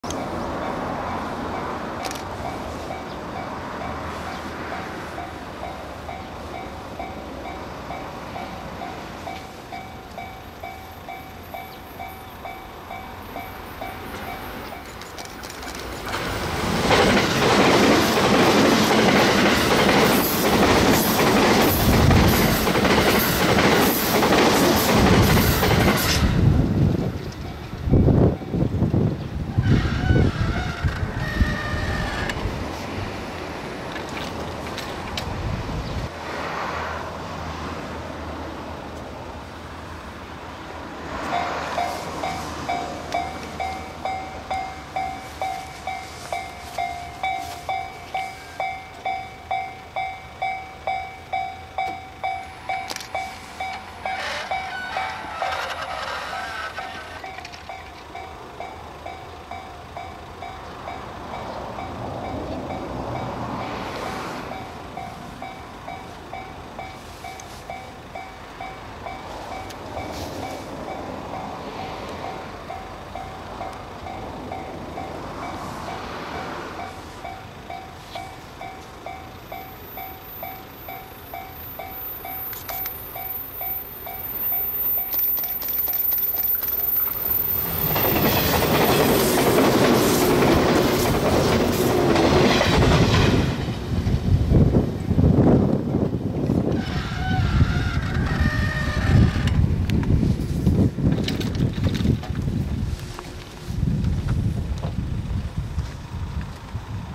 【4K定点撮影】中央本線（上諏訪～下諏訪）
JR中央本線の上諏訪～下諏訪間を通る列車を撮影しました。
E353系特急スーパーあずさ、189系回送が通過します。